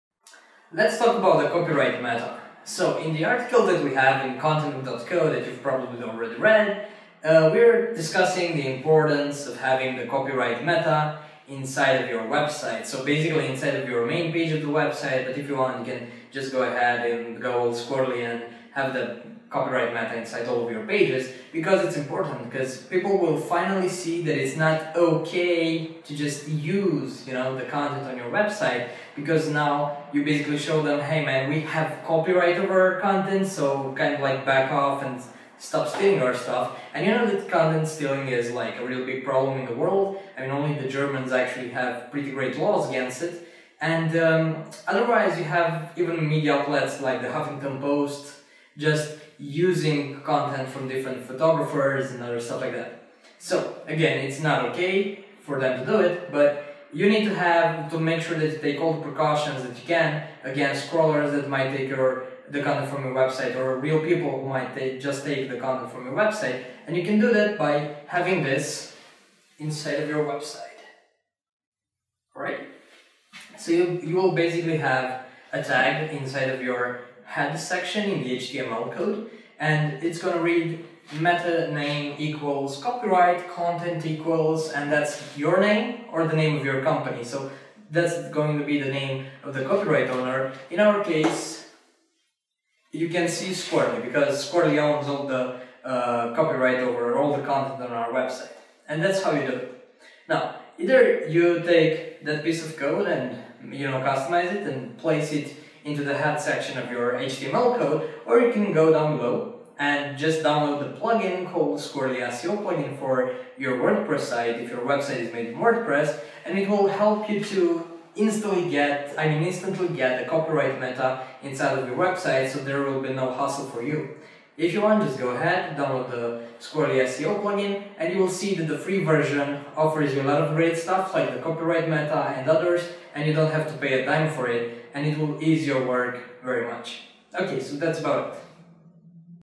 Meta Copyright - How to add the Copyright Meta Tag on your website
How do you explain that the photos and the text on your website are protected? You use the so called COPYRIGHT meta tag.
Now everybody can copy anything on the internet.

You may add this copyright meta tag to all of your webpages, so not only in the first index page.
 You can use multiple copyright meta tags if different parts of the content have different copyright information.

The Copyright Meta, as I said, keeps all your website content protected. So, if you added this feature on your website, you will have proof against someone that tries to copy a text or steal an image.
Although not many search engines look specifically for the Copyright META tag, it clearly specifies the terms of ownership or licensing arrangements directly in the headers of your webpage. Generally, search engines do not read and/or store this information.